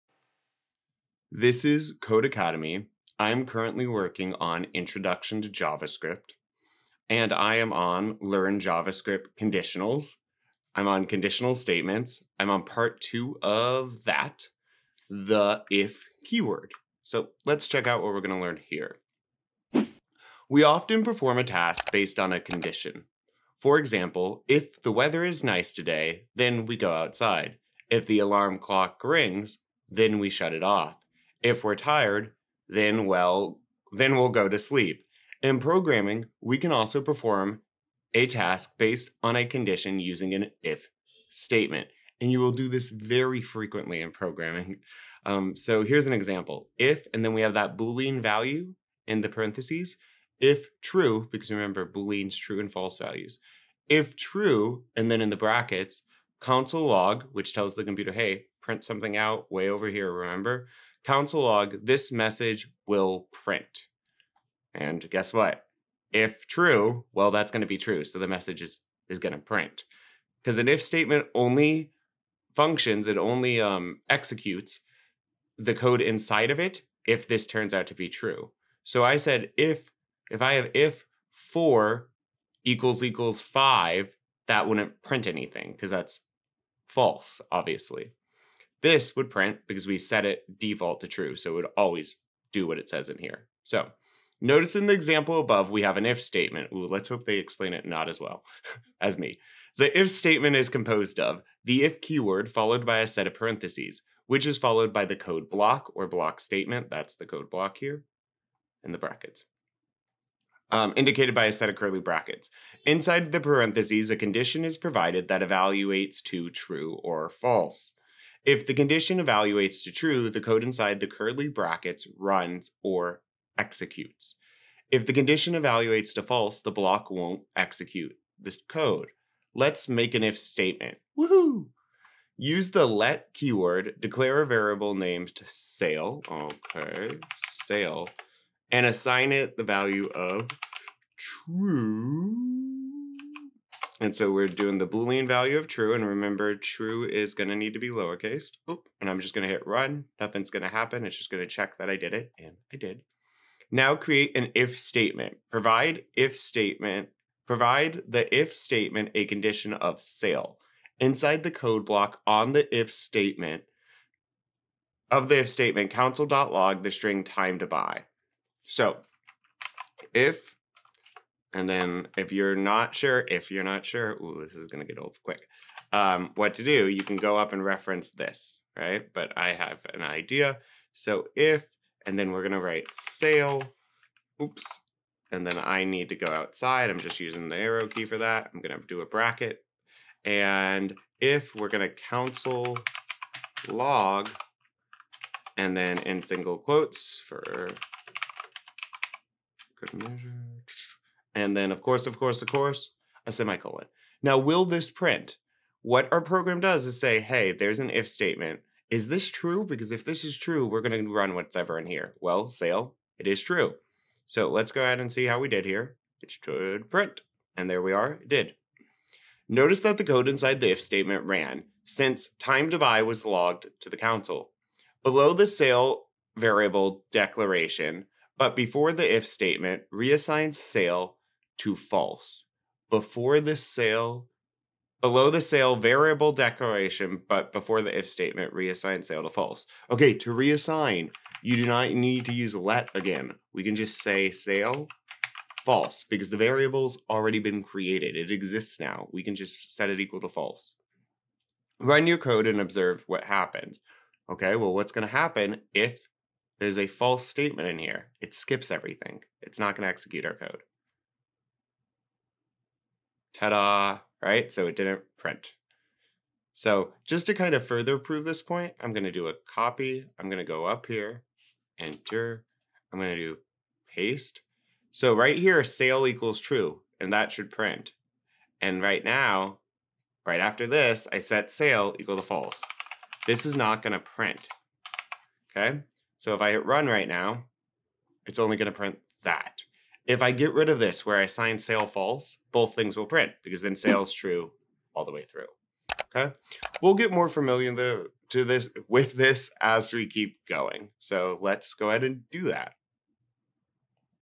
The if keyword Exercise 6 Conditional Statements- Intro to JavaScript Codecademy
Technology tutorial, simple and quick video that walks you through step-by-step The if keyword Exercise 6 of Conditional Statements from Intro to JavaScript on Codecademy .com.